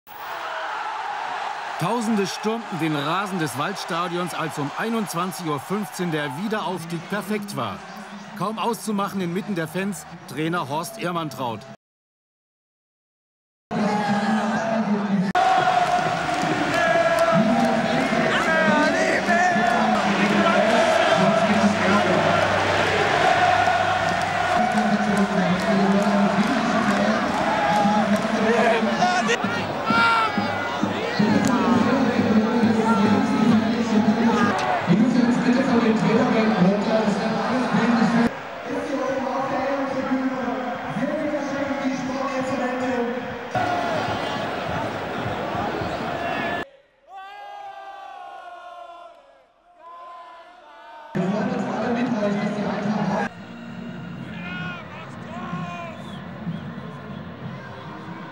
TBT | Aufstiegsfeier 1997/98
Happy Birthday, Alexander Kutschera! An seinem 51. Geburtstag blicken wir auf die Aufstiegsfeier nach dem letzten Spieltag 1997/98 zurück. In der Saison stand Kutschera in allen Pflichtspielen auf dem Platz.